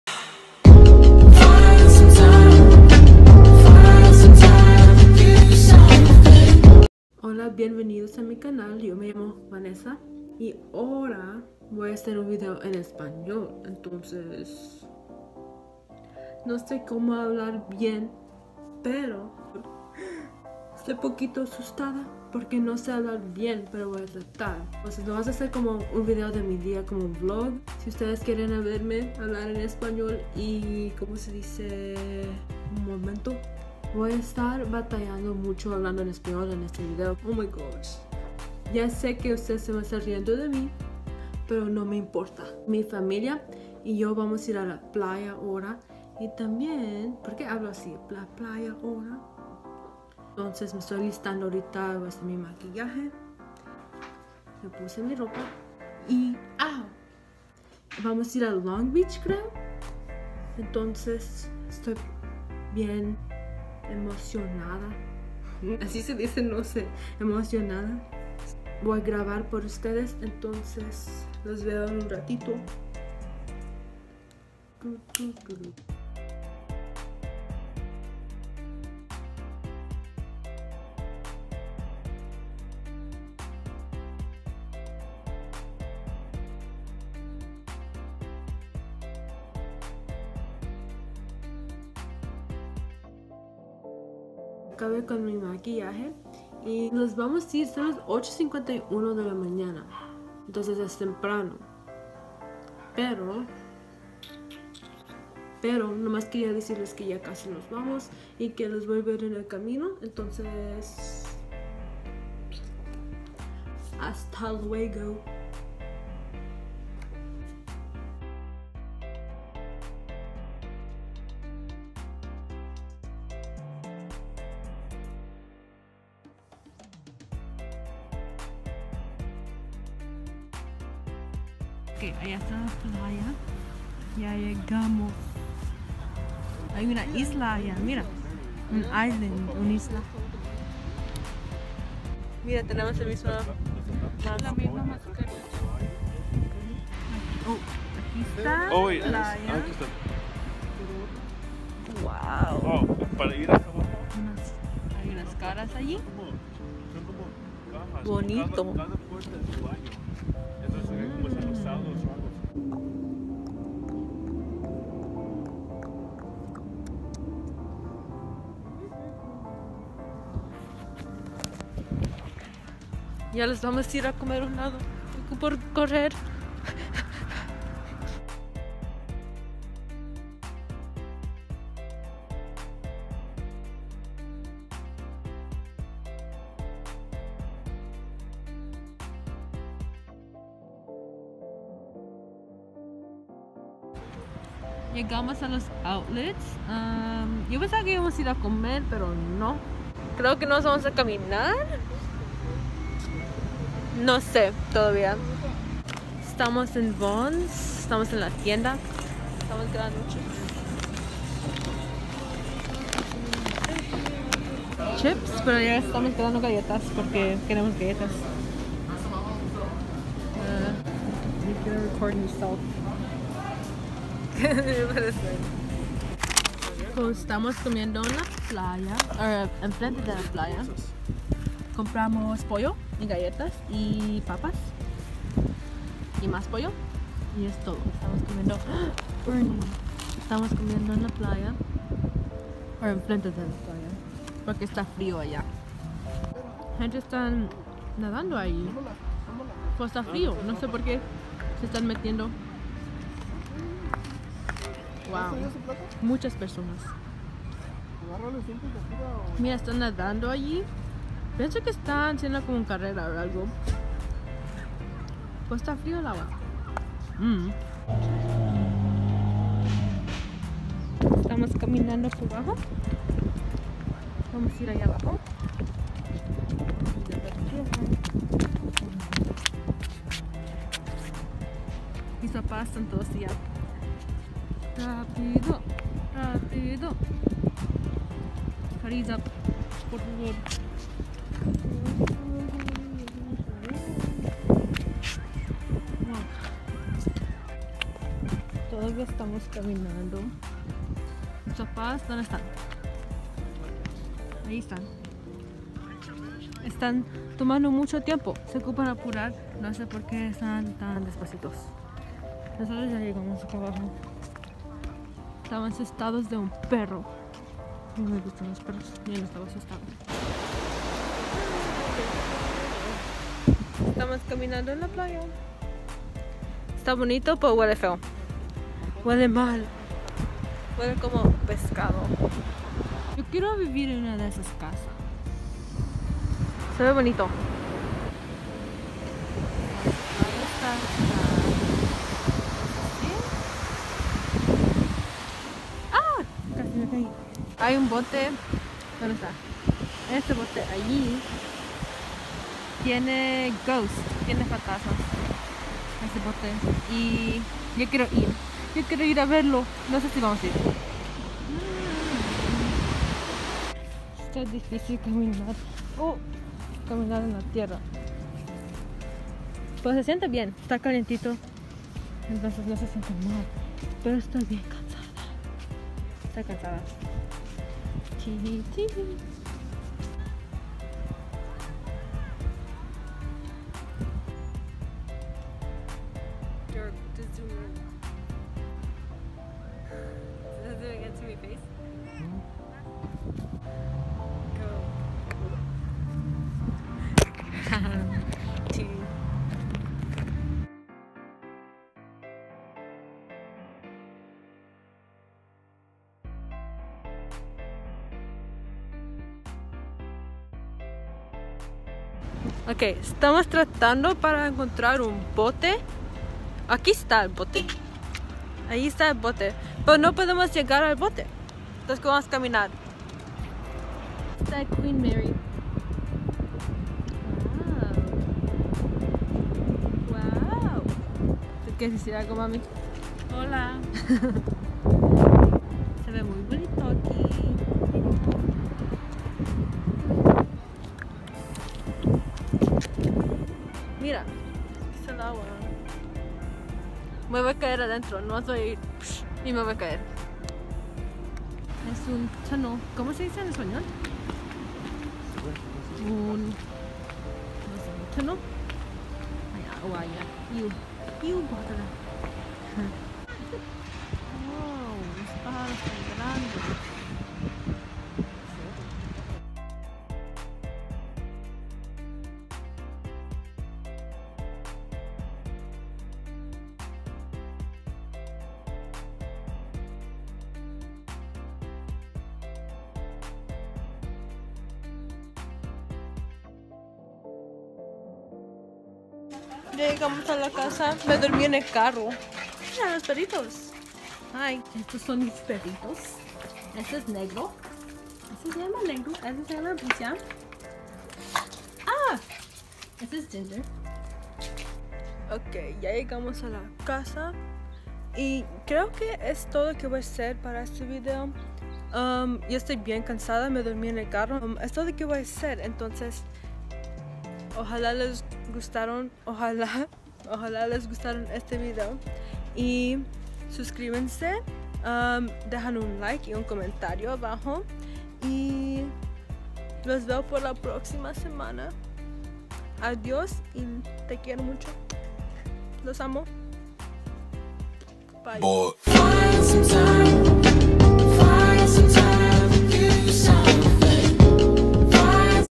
UN DIA EN MI VIDA! *si, hablo español*
Hola! Me llamo Vanesa y hoy los enseño mi dia con mi familia! Quería ser un video en español por todas las personas que hablan español. Gracias por viendo este video y ojalá que les gusto! :)

Que uso para gravar- IPhone 7+
Que uso para editar- Imovie
Que uso para editar mis thumbnails- Phonto
                                                                  PicsArt
                                                                  PhotoGrid